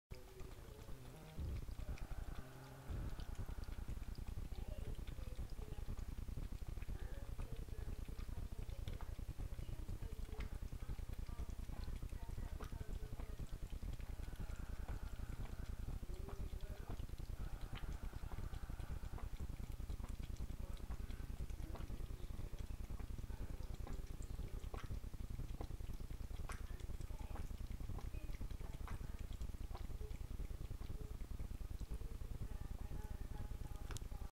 catfish.MP4
Puja, our persian cat, loves  water with a fish in it. She was never taught by her mother that fish are food, apparently. When I tried to stop her by lowering the water so she couldn't reach it, she pushed over the bowl, fish flopping on the floor, me scampering to clean it up. A real Garfield moment. From then on it was "Can I fill the bowl for you."